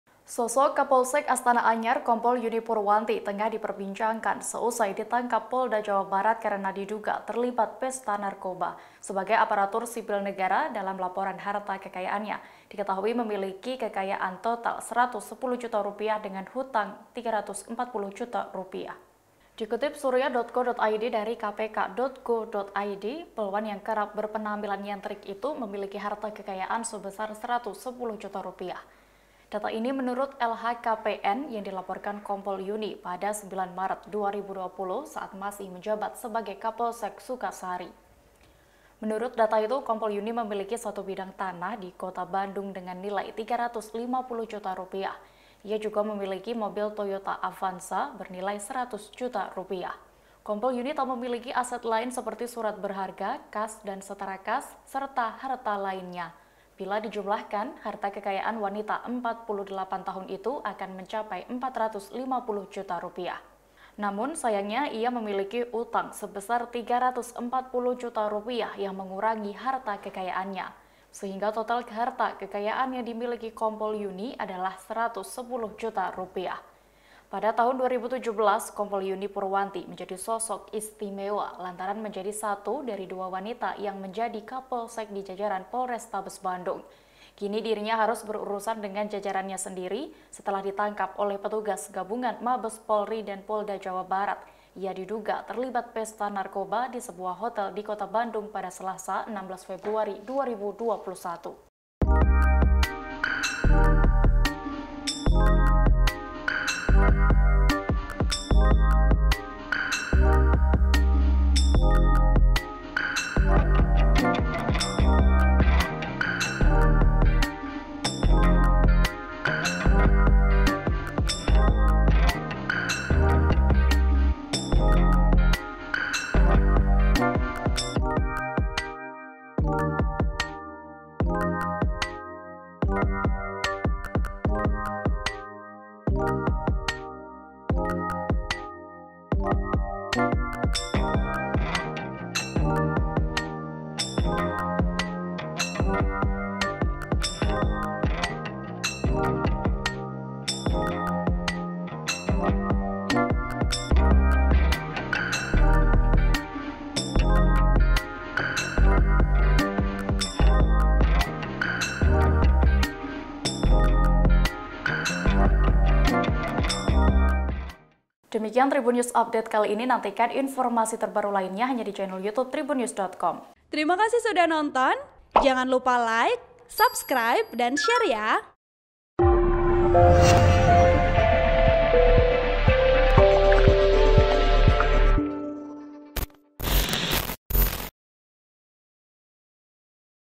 Sosok Kompol Yuni Purwanti Terduga Kasus Narkoba Punya Kekayaan Rp 110 Juta dengan Utang Rp340 Juta
TRIBUN-VIDEO.COM - Sosok Kapolsek Astana Anyar, Kompol Yuni Purwanti (49) tengah diperbincangkan seusai ditangkap Polda Jabar karena diduga terlibat pesa narkoba.

Sebagai aparatur sipil negara (ASN) dalam laporan harta kekayaannya diketahui memiliki kekayaan total Rp 110 juta dengan utang Rp 340 juta.

Dikutip Surya.co.id dari kpk.go.id, polwan yang kerap berpenampilan nyentrik itu memiliki harta kekayaan sebesar Rp 110 juta.

Data ini menurut LHKPN yang dilaporkan Kompol Yuni pada 9 Maret 2020 saat masih menjabat sebagai Kapolsek Sukasari.

Menurut data itu Kompol Yuni memiliki satu bidang tanah di Kota Bandung dengan nilai Rp 350 juta.

Ia juga memiliki mobil Toyota Avanza bernilai Rp 100 juta.

Kompol Yuni tak memiliki aset lain seperti surat berharga, kas dan setara kas, serta harta lainnya.

Bila dijumlahkan, harta kekayaan wanita 48 tahun itu akan mencapai Rp 450 juta.

Namun, ia memiliki utang sebesar Rp 340 juta yang mengurangi harta kekayaannya.

Sehingga total harta kekayaan yang dimiliki Kompol Yuni adalah Rp 110 juta.

Pada 2017 Kompol Yuni Purwanti menjadi sosok istimewa lantaran menjadi satu dari dua wanita yang menjadi Kapolsek di jajaran Polrestabes Bandung.

Kini dirinya harus berurusan dengan jajarannya sendiri setelah ditangkap oleh petugas gabungan Mabes Polri dan Polda Jabar.

Ia diduga terlibat pesta narkoba di sebuah hotel di Kota Bandung pada Selasa (16/2/2021). (Tribun-video.com/Surya.co.id)

Artikel ini telah tayang di surya.co.id dengan judul Sumber Kekayaan Kapolsek Astana Anyar Kompol Yuni Purwanti, Hartanya Ratusan Juta,
Penulis: Abdullah Faqih
Editor: Iksan Fauzi